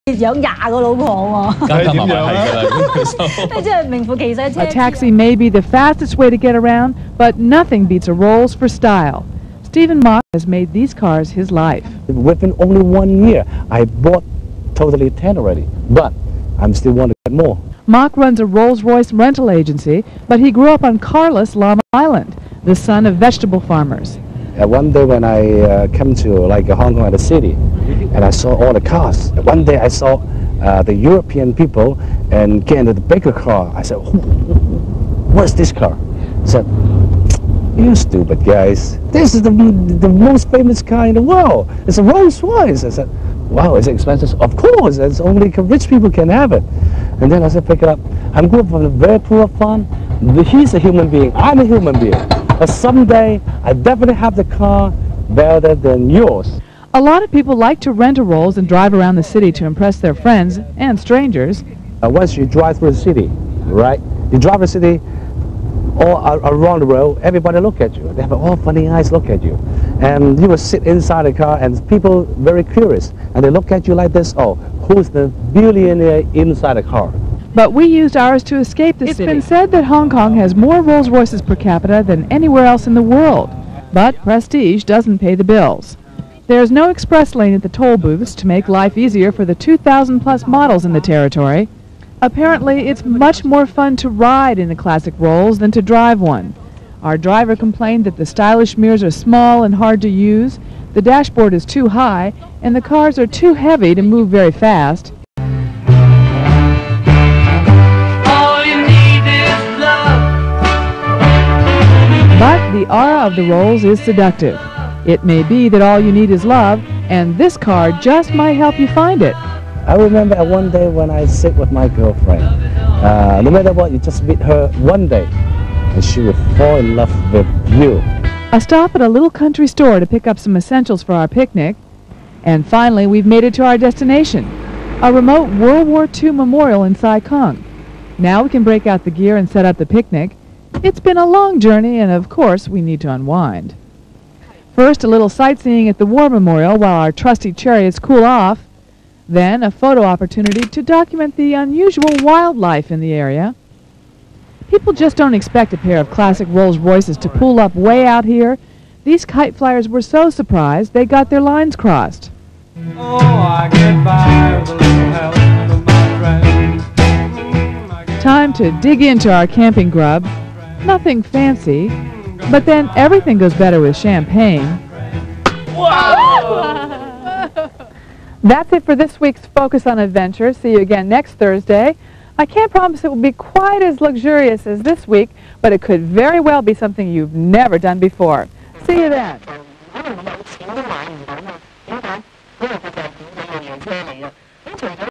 Classic Rolls 勞斯汽車旅遊服務有限公司
21週年回顧 環宇台英文訪問Classic Rolls Travel Service Co.Ltd 勞斯汽車旅遊服務有限公司#勞斯公司自1995年開業以來,已有超過萬對新人享受過本­司提供的優質花車服務,並一直深受各界人士支持及喜愛，希望將更完美的服務承送大家！